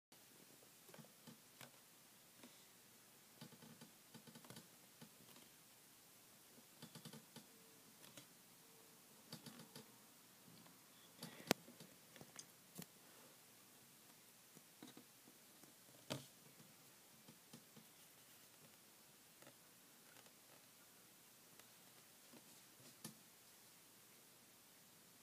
Can You Guess Which MSM Monsters These Are? (Read description)
Not all of them in the game are purple, The only colour of clay I had was purple